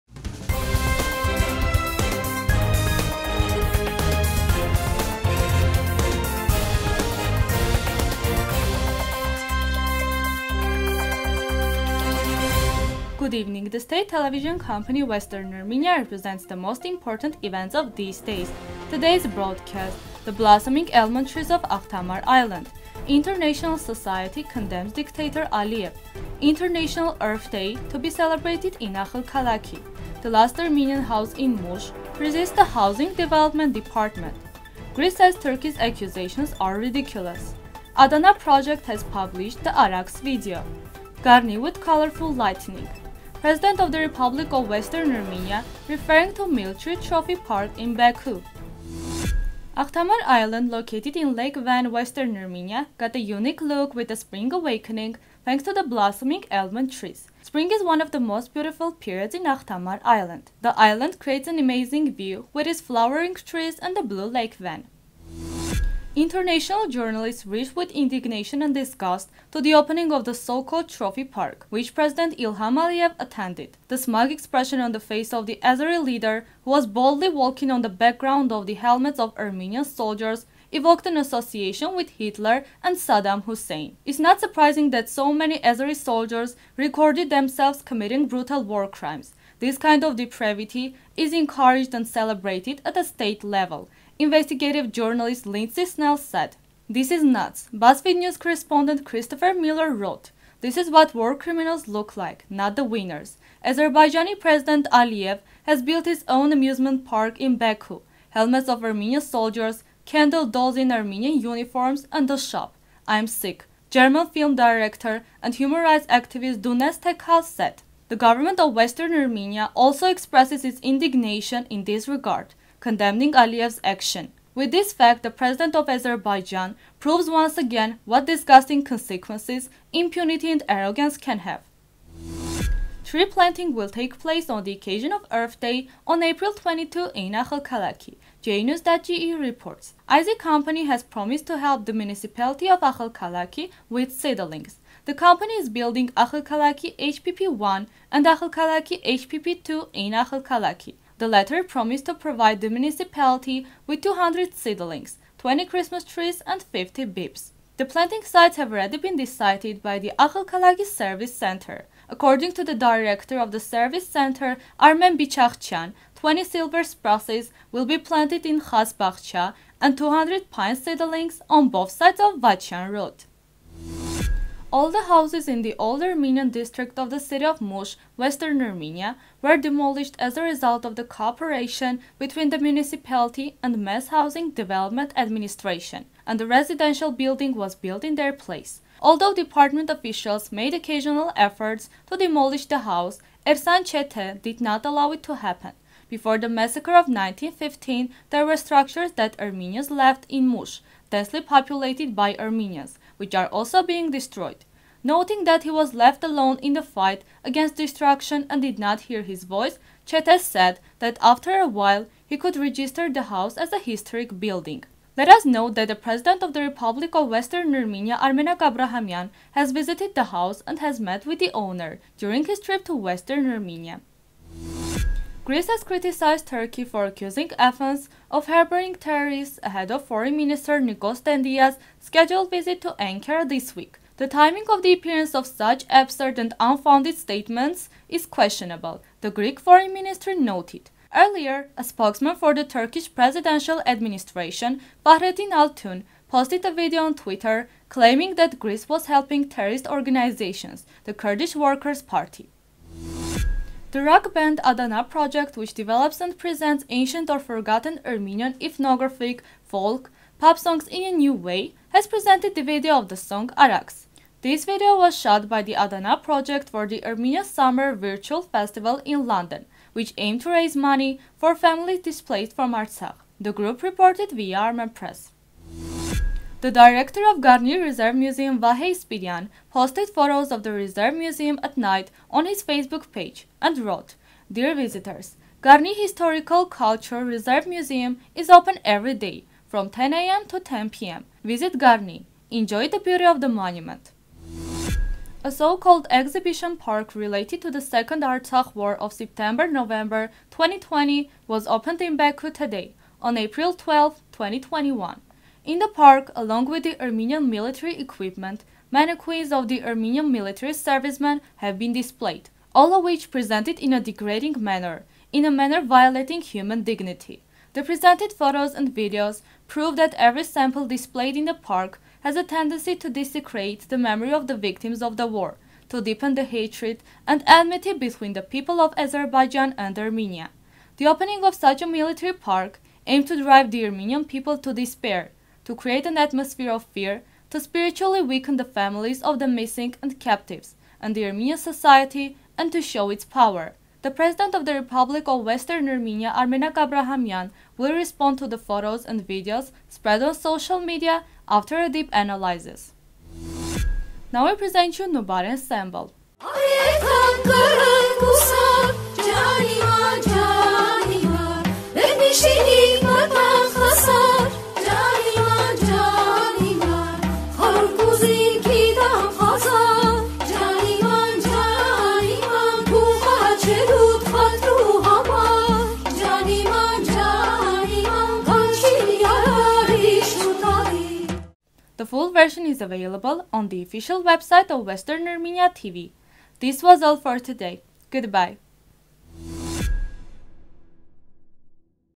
International society condemns ‘‘dictator Aliyev’’.News 13 04 2021
1.The blossoming almond trees of Akhtamar Island
2.International society condemns ‘‘dictator Aliyev’’
3.International Earth Day to be celebrated in Akhalkalaki
4.The last Armenian house in Mush resists the Housing Development Department
5.Greece says Turkey’s accusations are ridiculous
6.Adana Project has published the ‘‘Araks’’ video
7.Garni with colorful lightning
8.RWA President referring to ‘‘Military Trophy Park’’ in Baku